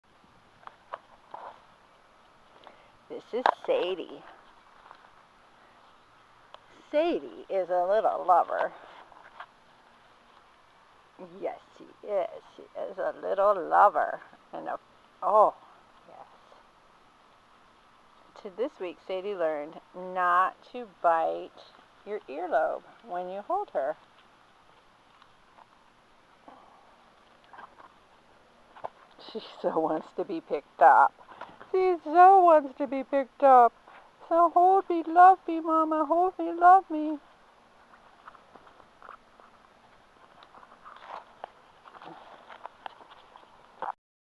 Sadie on Independence Day
Schipperke puppy playing.